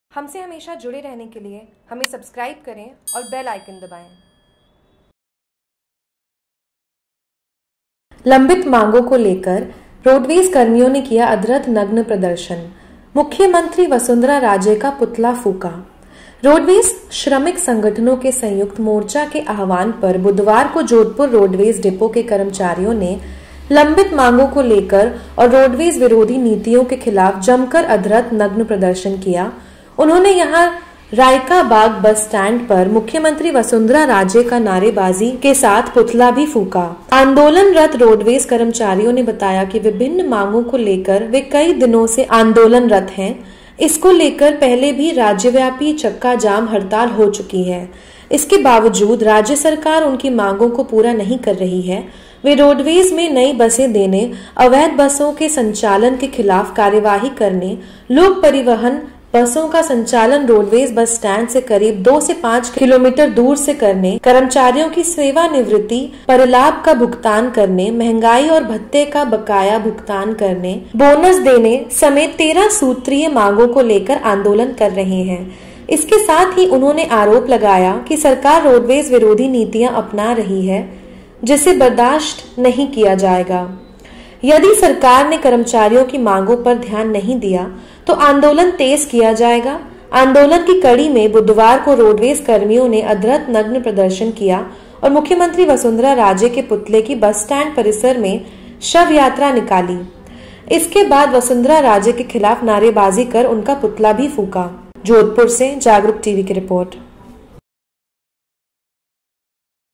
JAGRUK TV : जोधपुर : रोडवेज कर्मियों ने किया अद्र्ध नग्न प्रदर्शन
लंबित मांगों को लेकर रोडवेज कर्मियों ने किया अद्र्ध नग्न प्रदर्शन
मुख्यमंत्री वसुंधरा राजे का पुतला फूंका
जोधपुर। रोडवेज श्रमिक संगठनों के संयुक्त मोर्चा के आह्वान पर बुधवार को जोधपुर रोडवेज डिपो के कर्मचारियों ने लंबित मांगों को लेकर और रोडवेज विरोधी नीतियों के खिलाफ जमकर अद्र्ध नग्न प्रदर्शन किया। उन्होंने यहां राइका बाग बस स्टैण्ड पर मुख्यमंत्री वसुंधरा राजे का नारेबाजी के साथ पुतला भी फूंका।
आंदोलनरत रोडवेज कर्मचारियों ने बताया कि विभिन्न मांगों को लेकर वे कई दिनों से आंदोलनरत है। इसको लेकर पहले भी राज्यव्यापी चक्काजाम हड़ताल हो चुकी है। इसके बावजूद राज्य सरकार उनकी मांगों को पूरा नहीं कर रही है। वे रोडवेज में नई बसें देने, अवैध बसों के संचालन के खिलाफ कार्रवाई करने, लोक परिवहन बसों का संचालन रोडवेज बस स्टैंड से करीब 2 से 5 किमी दूर से करने, कर्मचारियों की सेवानिवृति परिलाभ का भुगतान करने, महंगाई व भत्ते का बकाया भुगतान करने, बोनस देने समेत 13 सूत्रीय मांगों को लेकर आंदोलन कर रहे है। इसके साथ ही उन्होंने आरोप लगाया कि सरकार रोडवेज विरोधी नीतियां अपना रही है जिसे बर्दाश्त नहीं किया जाएगा। यदि सरकार ने कर्मचारियों की मांगों पर ध्यान नहीं दिया तो आंदोलन तेज किया जाएगा। आंदोलन की कड़ी में बुधवार को रोडवेज कर्मियों ने अद्र्ध नग्न प्रदर्शन किया और मुख्यमंत्री वसुंधरा राजे के पुतले की बस स्टैंड परिसर में ही शवयात्रा निकाली। इसके बाद वसुंधरा राजे के खिलाफ नारेबाजी कर उनका पुतला फूंका।